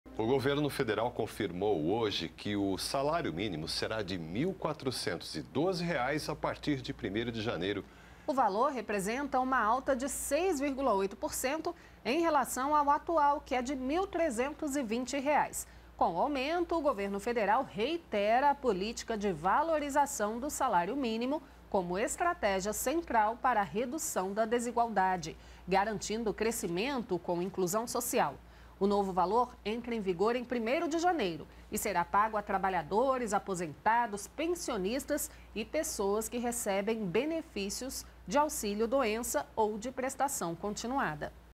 GOVERNO CONFIRMA QUE SALÁRIO MÍNIMO SERÁ DE R$ 1.412 À PARTIR DE 1º DE JANEIRO
O governo federal confirmou que o salário mínimo será de R$ 1.412 a partir de primeiro de janeiro. O valor representa uma alta de 6,8% em relação ao atual, que é de R$ 1.320. Com o aumento, o Governo Federal reitera a política de valorização do salário mínimo como estratégia central para a redução da desigualdade, garantindo crescimento com inclusão social. O novo valor entra em vigor em 1º de janeiro e será pago a trabalhadores, aposentados, pensionistas e pessoas que recebem benefícios de auxílio-doença ou de prestação continuada.